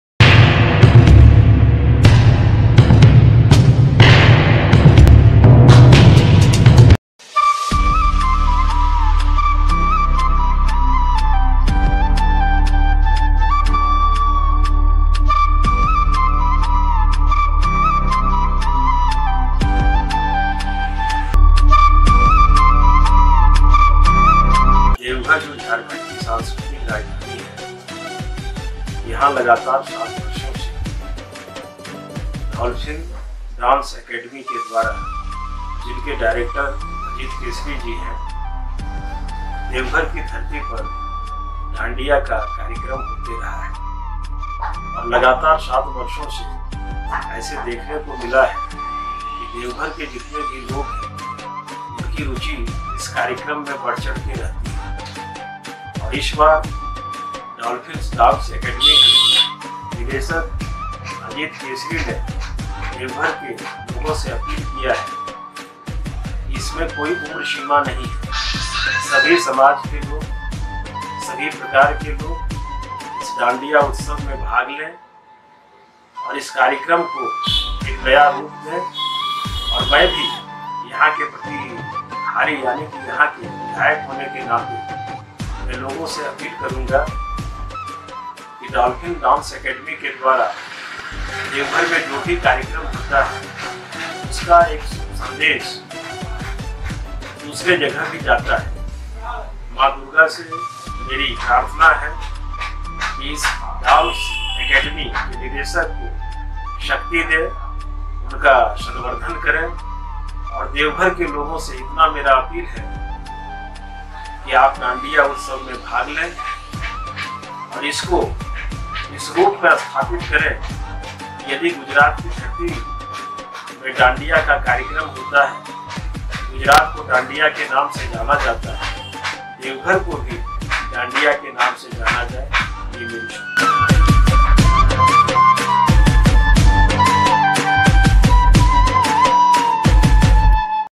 Deoghar Dandiya Utsav Spotted By Sri Narayan Das (विधायक) Of Deoghar
Spacel Thanks For:-

(Chandra films & Event Planner)

Editing By :-

Videography By:-

 Dance video

Choreographed By :-
Ajit keshri (Director of Dolphin Dance Academy)